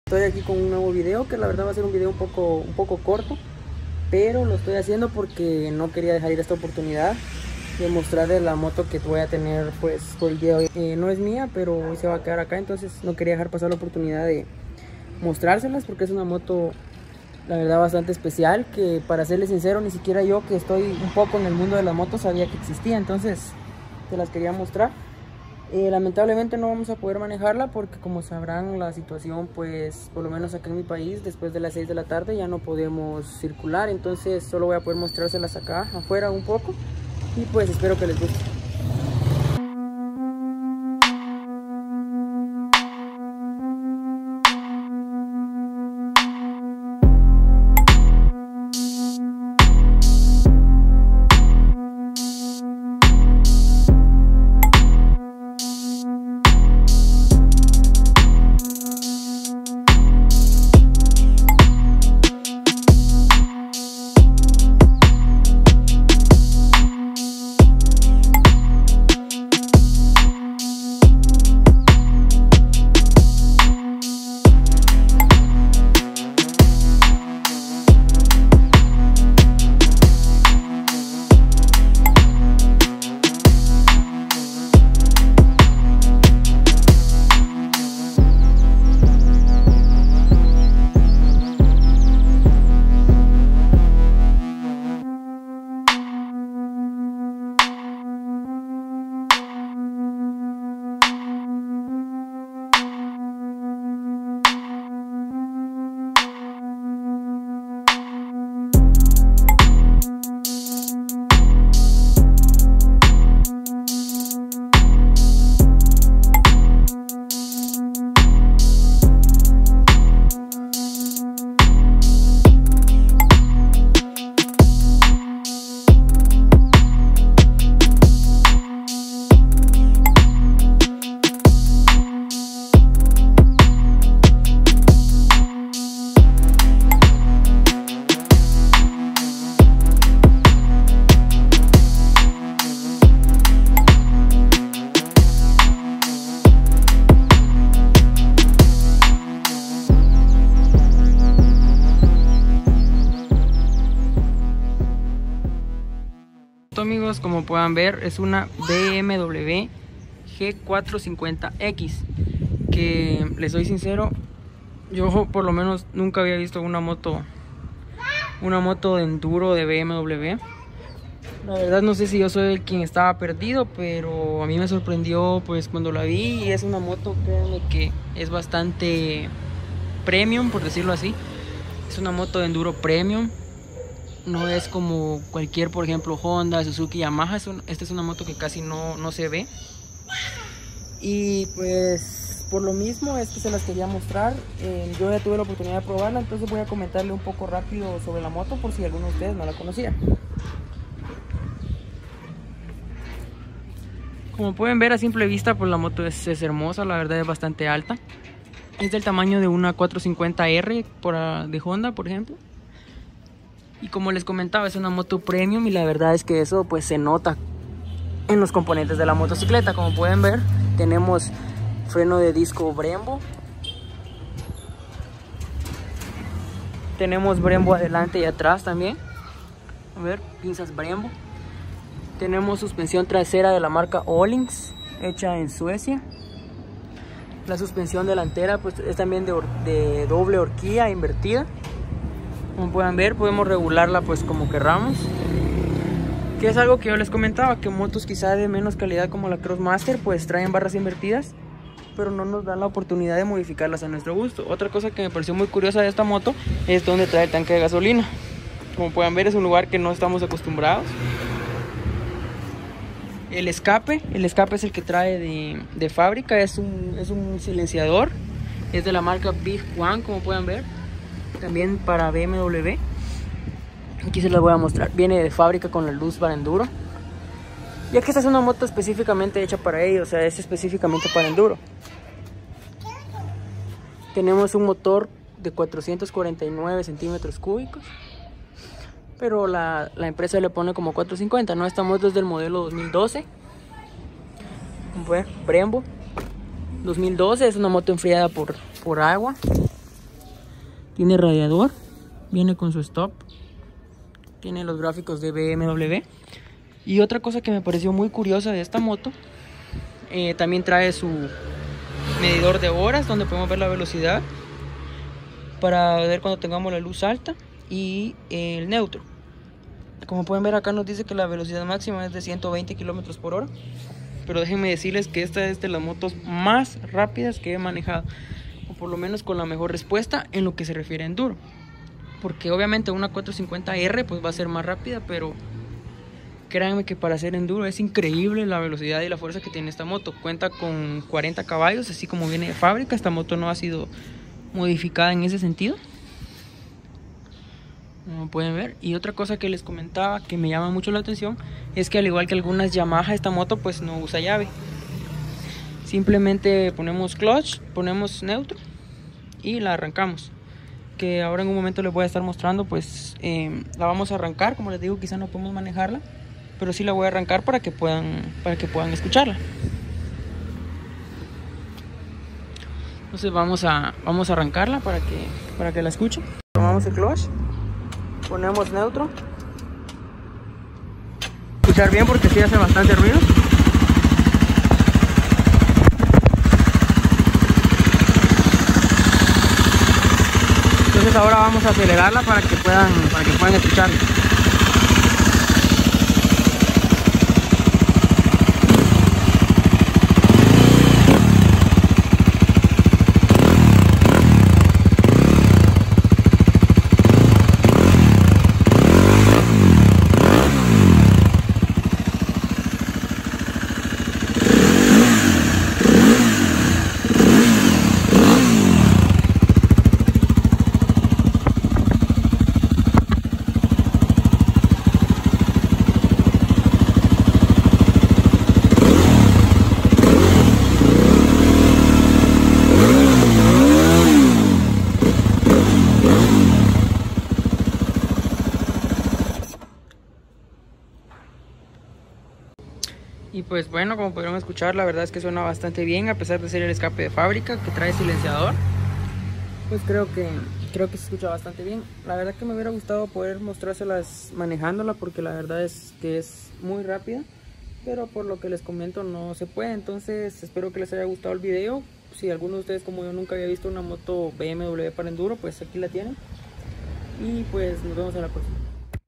La enduro de BMW / G 450X
Hola amigos, en este vídeo les traigo una motocicleta que me sorprendió muchísimo es una de las motos para enduro, mas exclusivas y únicas del mercado, traigo para ustedes la BMW G 450X. ¿Será esta la mejor moto para enduro?